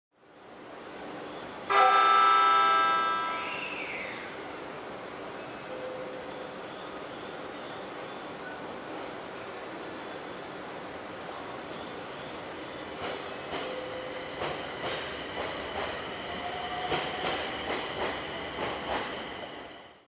2017/5/13(Sat) 青い森鉄道 703系 ミュージックホーン
2017/5/13(Sat)、青森駅に到着した587Mが回送のため発車する様子です。